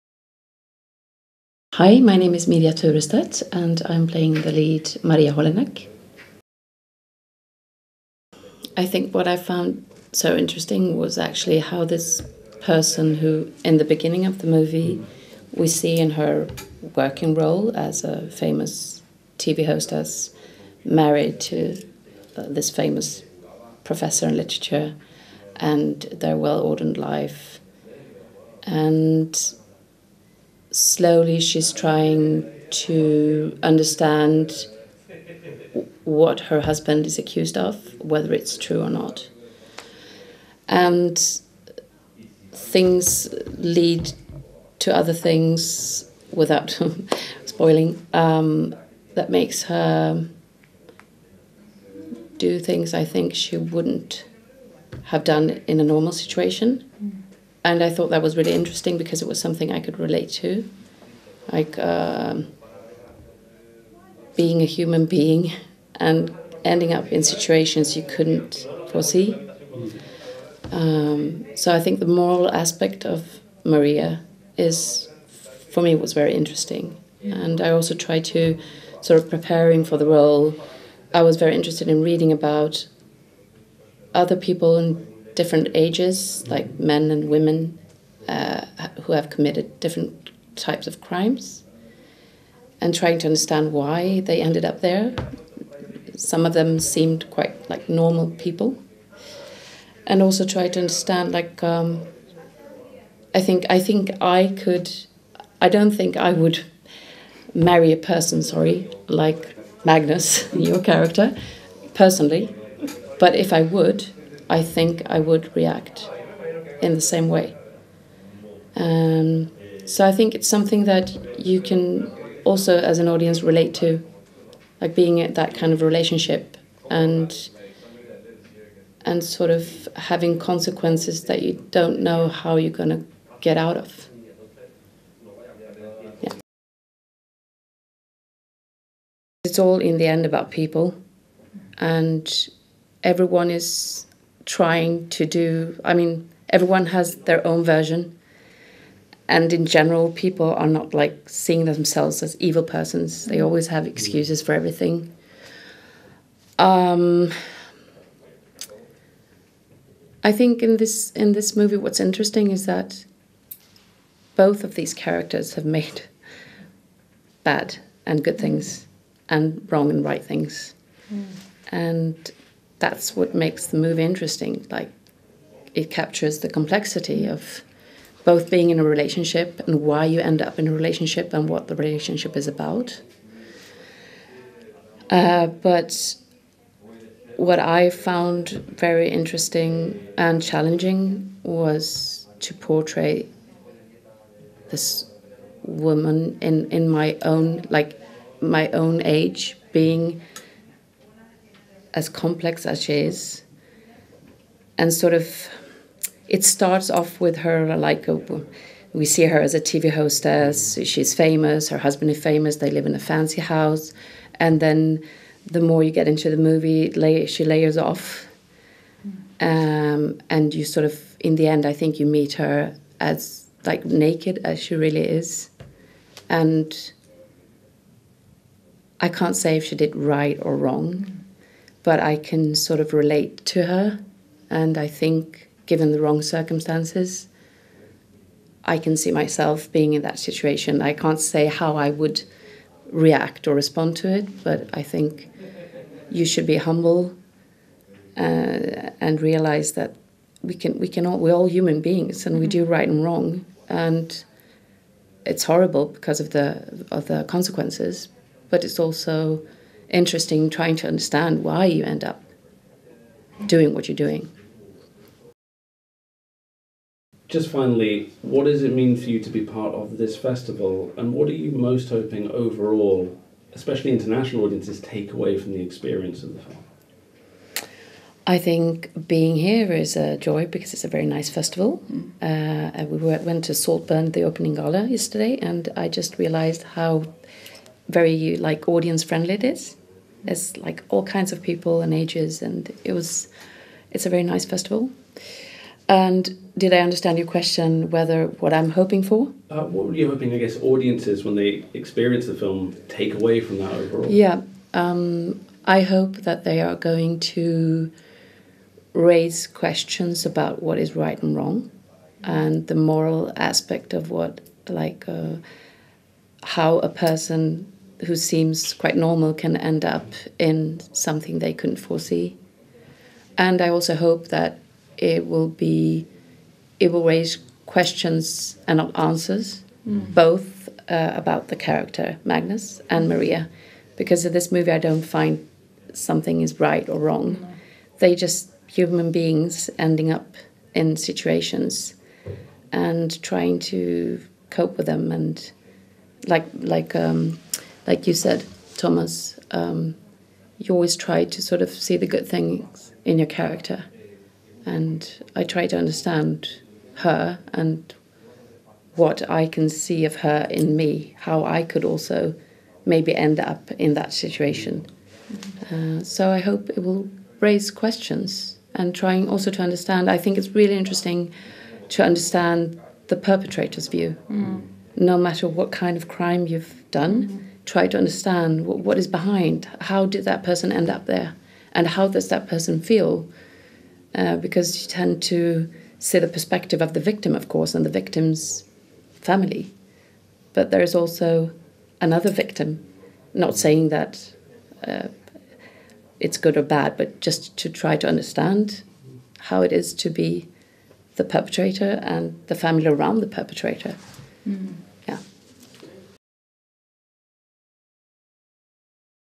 Unmoored Mirja Turestedt Interview
Red Caret News TV sits down with Unmoored director Caloine Ingvarsson alongside  stars Thomas W. Gabrielsson and Mirja Turestedt for an in depth chat about the  intense drama during the BFI London Film Festival.

Red Carpet News TV provides extensive coverage of the BFI London Film Festival 2023, featured films and interviews include